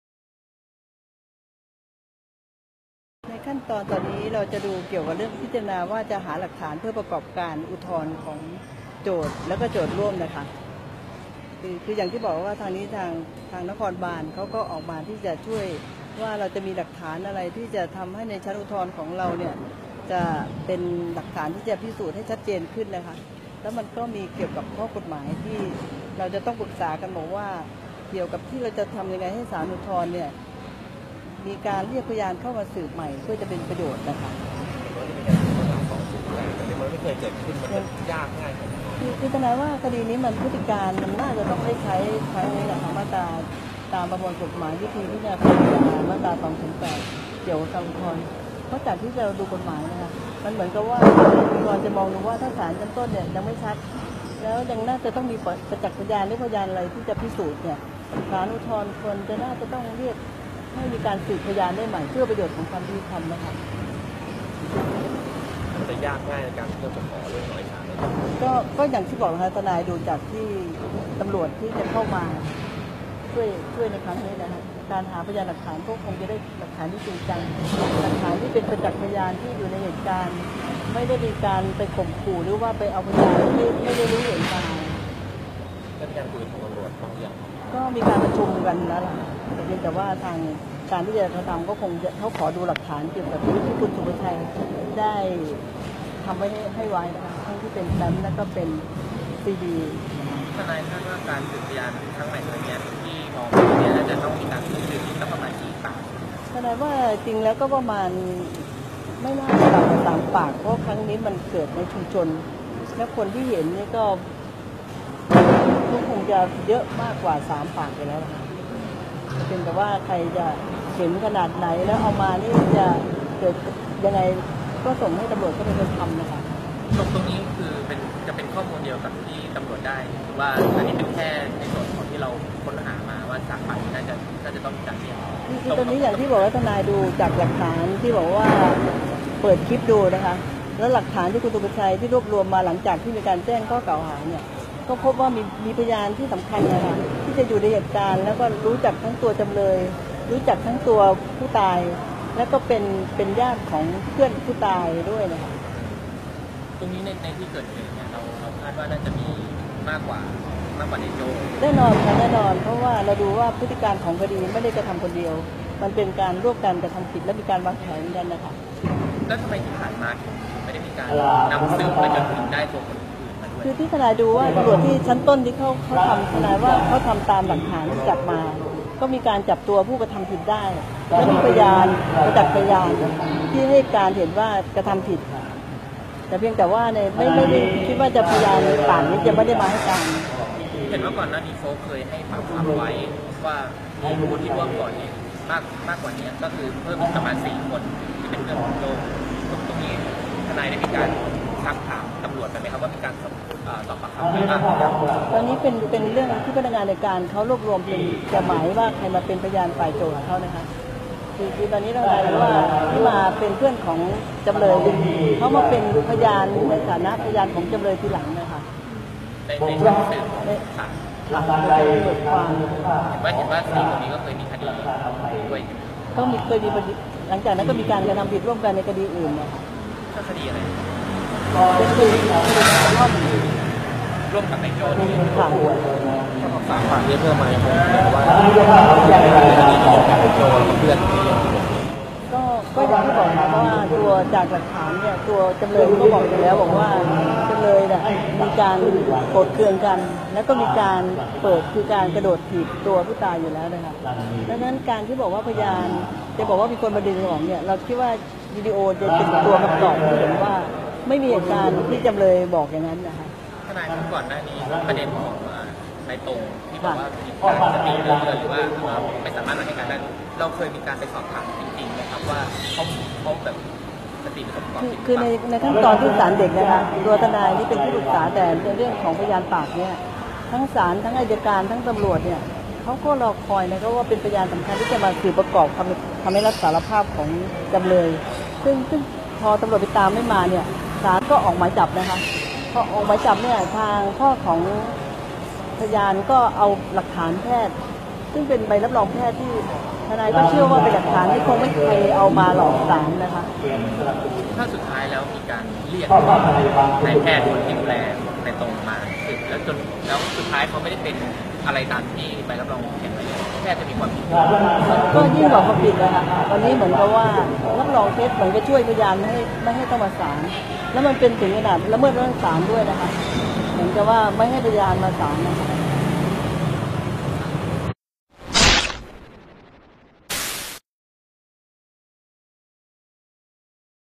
จ่อยื่นศาลอุทธรณ์ สืบพยานใหม่คดีแทงเต้ l อีจัน Ejan
นางสาวธนพร ศิริบานเย็น ทนายความ กล่าวว่า ขั้นตอนต่อจากนี้ คือการหาหลักฐานเพื่อการประกอบการพิจารณาอุทธรณ์ของทางโจทก์และโจทก์ร่วม เตรียมยื่นขอให้ศาลอุทธรณ์ เรียกพยานเข้ามาสืบใหม่ เพื่อจะเป็นประโยชน์ เพราะตามประมวลกฎหมายวิธีพิจารณาคดีอาญา มาตรา 208 เกี่ยวกับการอุทธรณ์ เพราะจากที่เราดูกฎหมายแล้ว

ทางศาลอุทธรณ์จะมองดูว่าถ้าศาลชั้นต้นยังไม่ชัดแล้วน่าจะต้องมีประจักษ์พยานหรือพยานอะไรที่จะต้องพิสูจน์ก็จะต้องเรียกให้มีการสืบพยานได้ใหม่ เพื่อประโยชน์ของความยุติธรรม ซึ่งคาดว่าพยานที่จะสืบใหม่ในครั้งนี้ไม่น่าจะต่ำกว่า 3 ปาก เพราะว่าเป็นเหตุการณ์ที่เกิดในที่ชุมชนปละจากคลิปวีดีโอกล้องวงจรปิด จึงคิดว่าน่าจะมีพยานสำคัญที่อยู่ในเหตุการณ์และรู้จักทั้งตัวจำเลยและผู้ตายอีกหลายคน